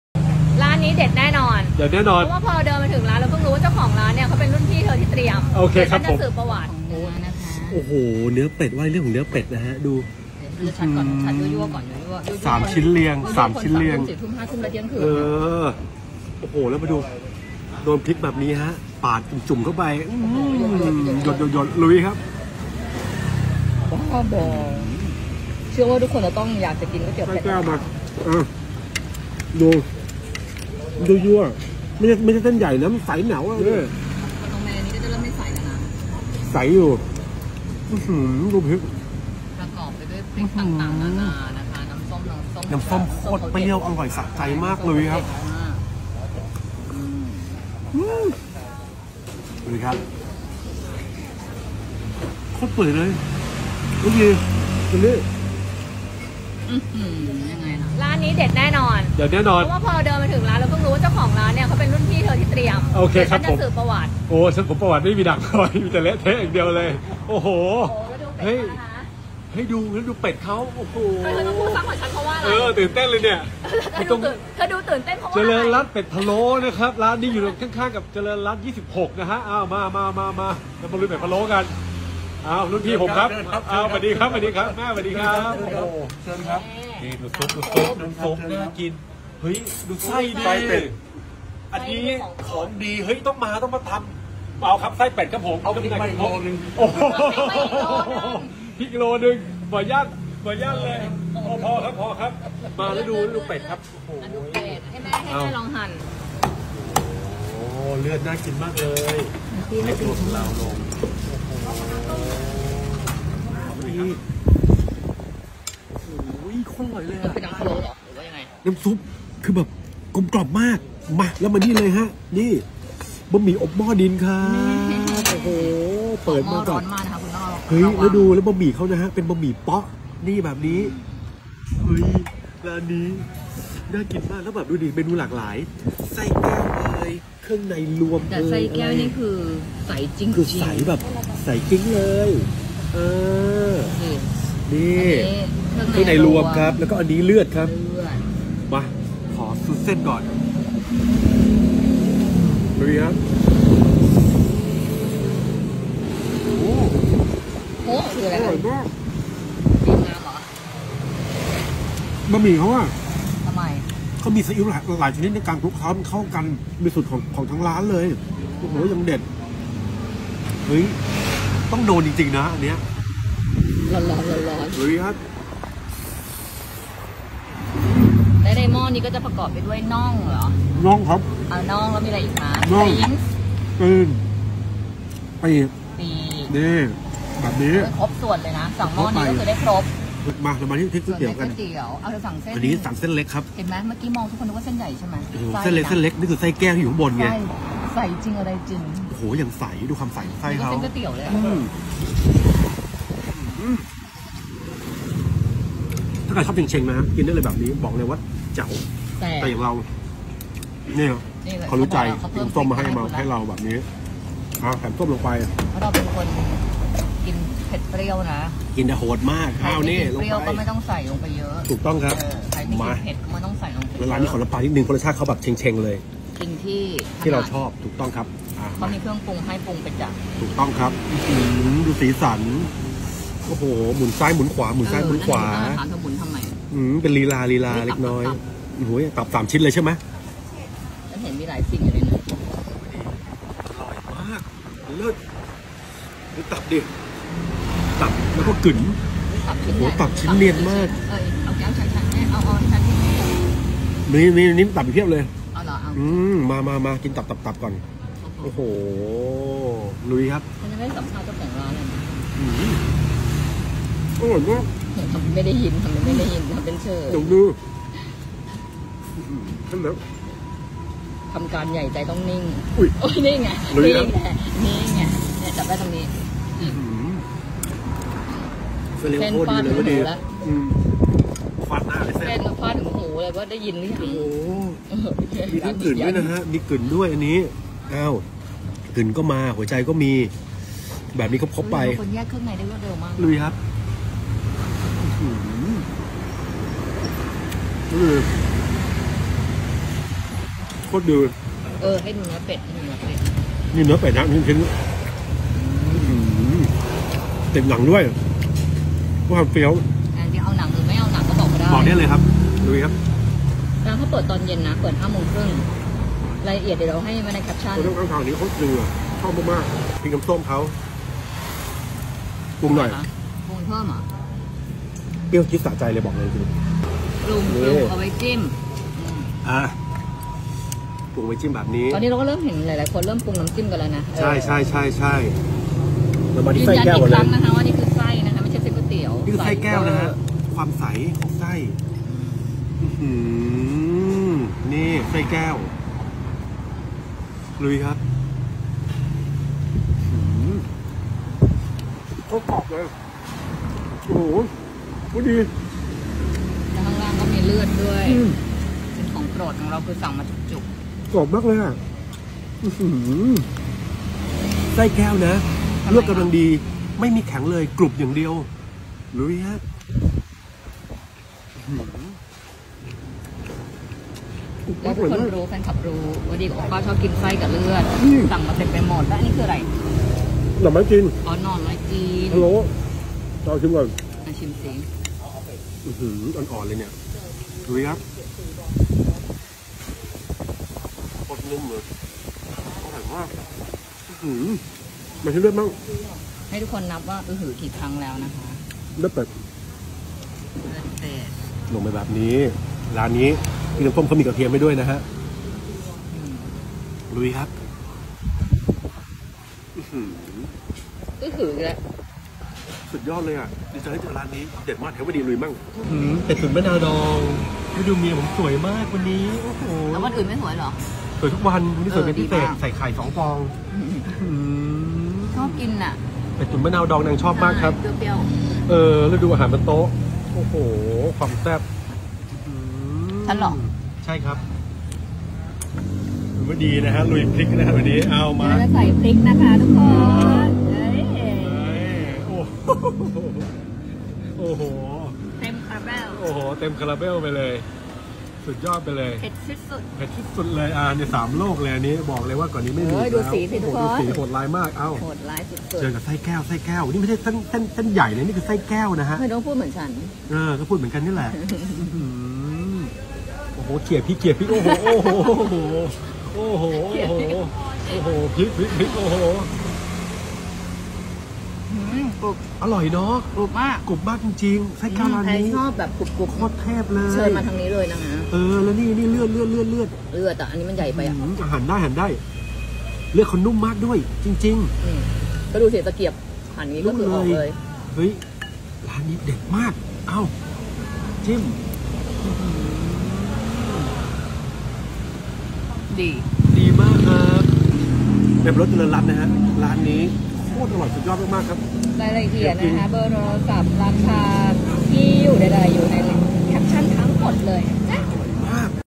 สายก๋วยเตี๋ยวเป็ดห้ามพลาด เป็ดนุ่ม ไส้แก้วดีสุดๆ | MAWIN FINFERRR
อปป้าเปิด YouTube Membership แล้วน้า มีอีโมจิน่ารักๆให้ใช้ด้วย มาสมัครกันเยอะๆนะ🥰

ร้านนี้อร่อยมากกกกก
ใครสายก๋วยเตี๋ยวเป็ดห้ามพลาด
ไส้แก้ว ดีสุดๆ เลือดก็นุ่ม
บะหมี่อบหม้อดินก็เยี่ยมมม 😋
ร้าน : เจริญรัถเป็ดพะโล้
พิกัด : เจริญรัถ 26
เมนู : เป็ดล้วนๆ
ราคา : 60 บ. ขึ้นไป
เวลา : 17:30-22:00 น.

ติดตามอปป้าผู้สรรหาอาหารเด็ดมาจุดประกายความหิวยามค่ำคืนได้ที่